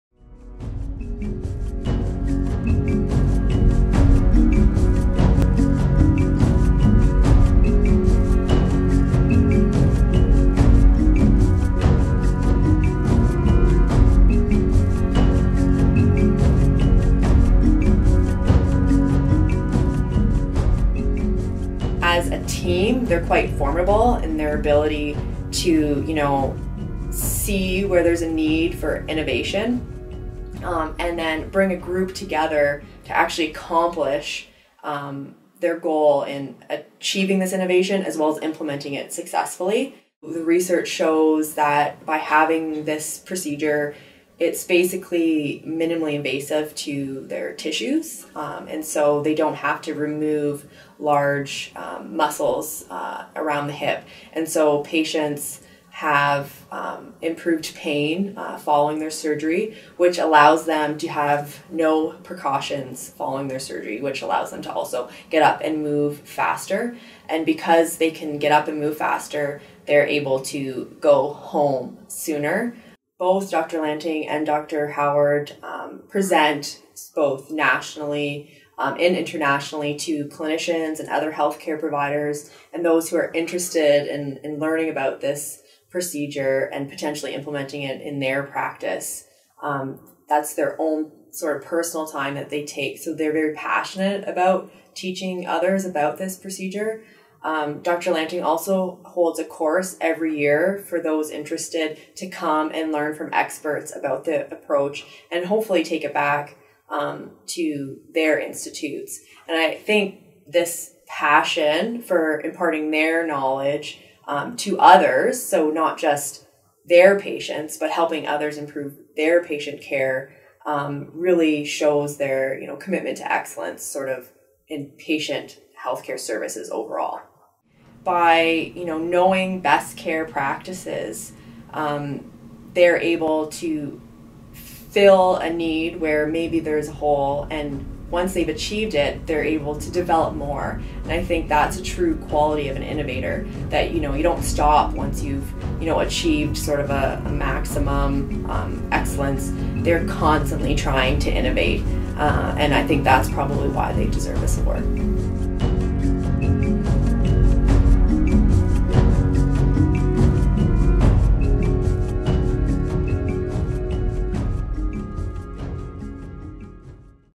2016 President’s Award – Innovation - Drs. Howard and Lanting
Dr. Brent Lanting and Dr. James Howard  are the recipients of the 2016 President’s Award – Innovation. Listen to Lyndsay Somerville, one of their nominators, discuss why they are deserving of the award.